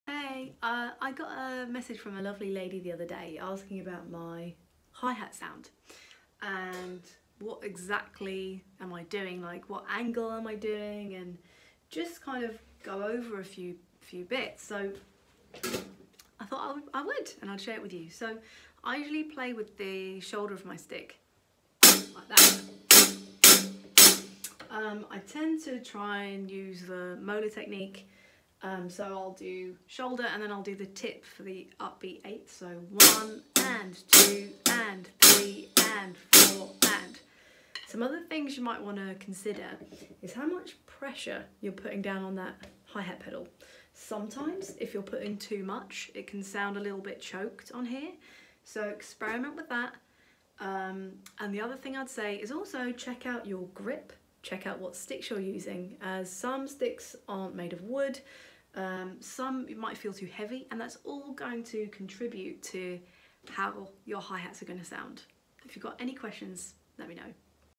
Tips for improving your hihat sound
A very quick video on a few things to consider when wanting to improve your hihat sound.